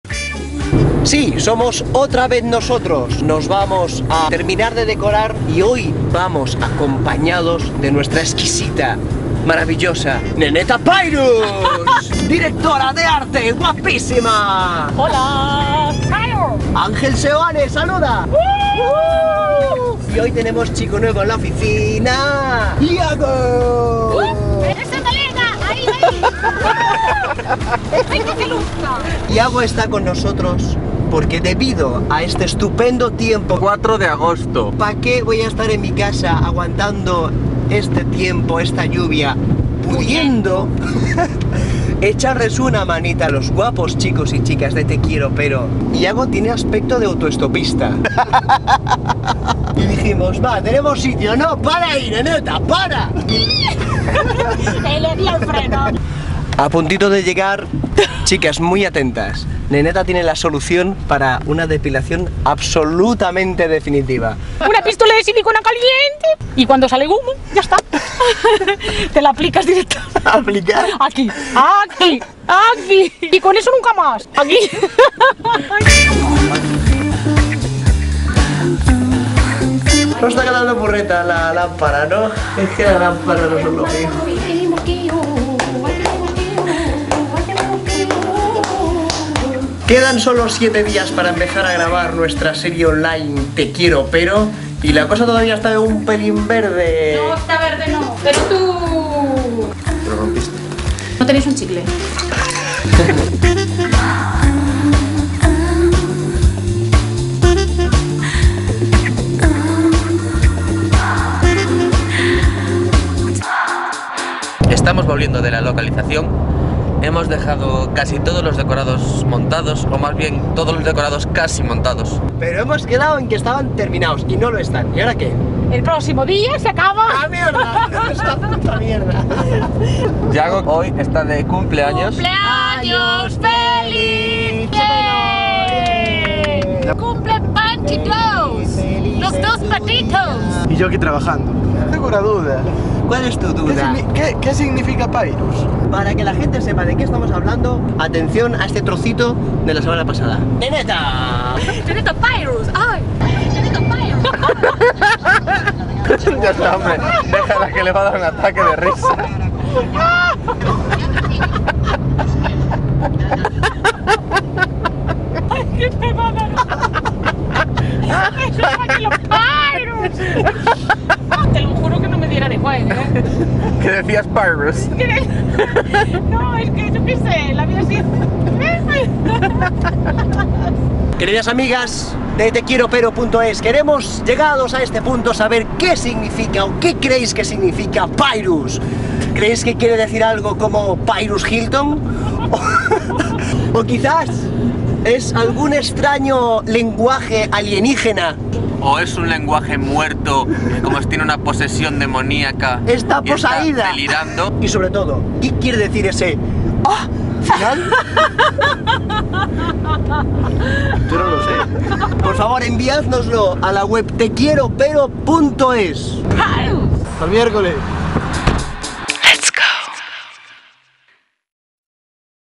Terminando los decorados de TE QUIERO PERO
El equipo técnico de TE QUIERO PERO. Neneta, Ángel, Iago y Carlos se pone en marcha de nuevo para terminar (por fin) los decorados de la nueva serie online "TE QUIERO PERO". Faltra menos de una semana para empezar a rodar! Sigue todas las novedades en tequieropero.es